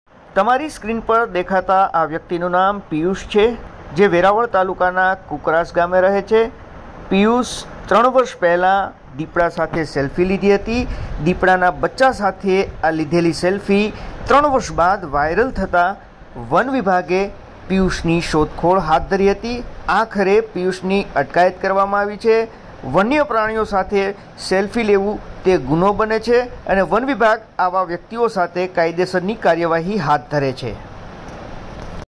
દીપડા ના બચ્ચા ની સેલ્ફી લેવી ભારે પડી
દીપડા ના બચ્ચા સાથે સેલ્ફી લેવી કુકરાશ ના વ્યક્તિ ને ભારે પડી
lepord dipdo selfi forest gir somnath,short